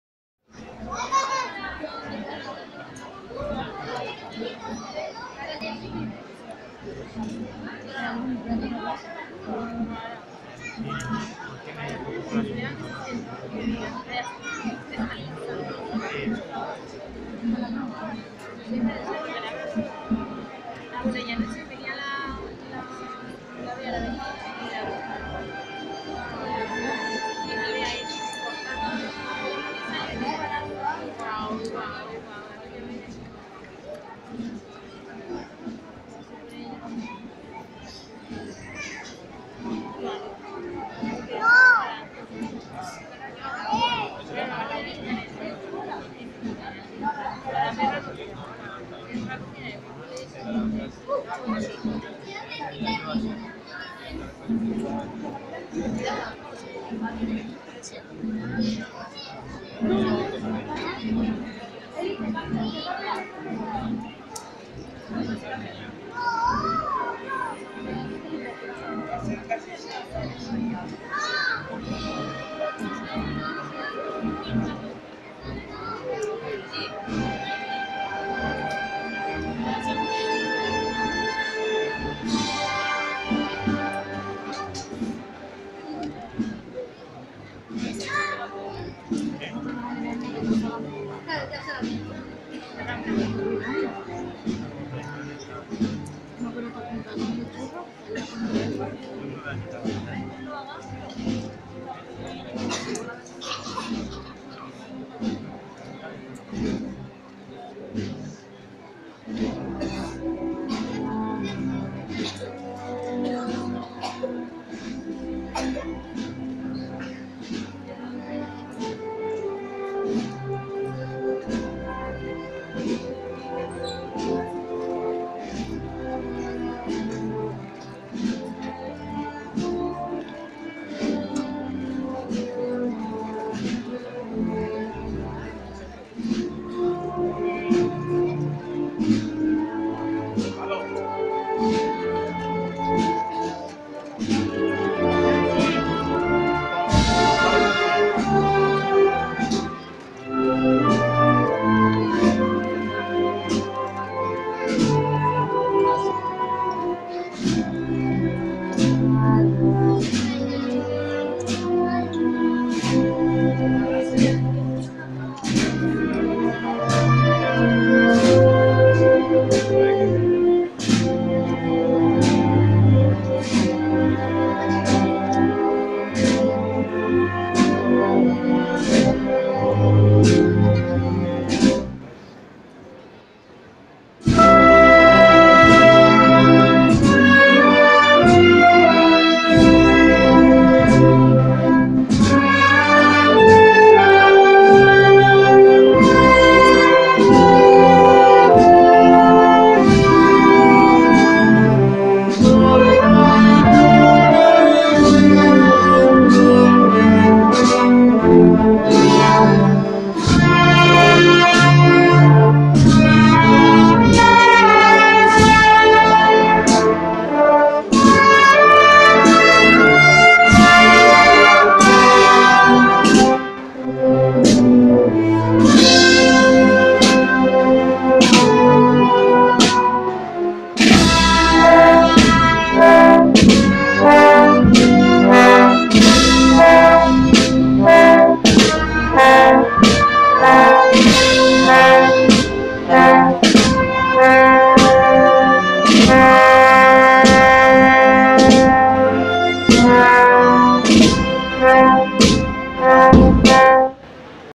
Martes Santo 2018 de Granada: Soledad de Ntra. Sra por San Matías
Cofradía del Señor de la Humildad, Soledad de Nuestra Señora y del Dulce Nombre de Jesús. Conocida popularmente como la Cañilla.